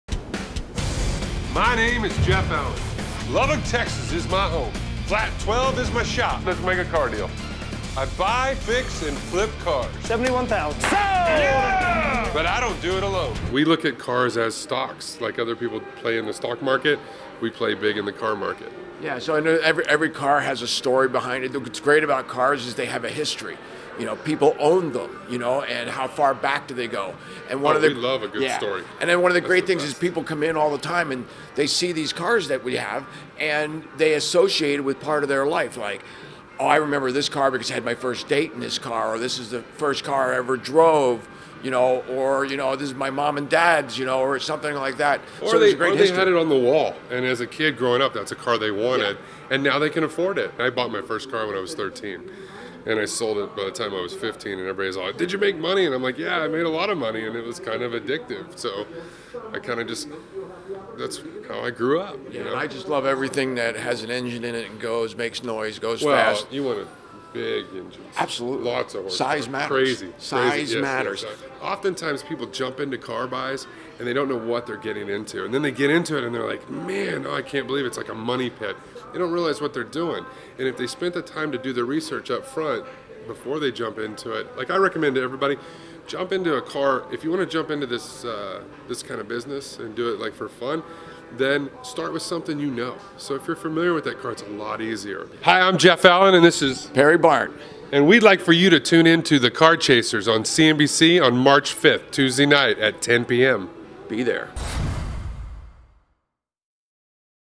"The Car Chasers" Premieres Tonight on CNBC Prime
Tonight at 10pm ET/PT "The Car Chasers" premieres on CNBC Prime. The show focuses on Jeff Allen and Perry Barndt who make their living  traveling the country to buy and sell classic and exotic cars. Whether it's a rare Shelby Mustang or a vintage hot rod, the key is buy low and sell high...which doesn't always happen.

Also in the mix is Jeff's dad, Tom Souter who runs a classic car dealership right around the corner from Jeff's shop in Lubbock, Texas. They are not just regular trading partners; they are trading partners hell-bent on one-upmanship. Tom says doing a deal with his son is like being locked in a closet with a porcupine, "it's gonna hurt but you know it won't kill you."

We chatted with Jeff and Perry about their business and they gave us their tips for car buying. Check it out here, and don't forget to tune in tonight!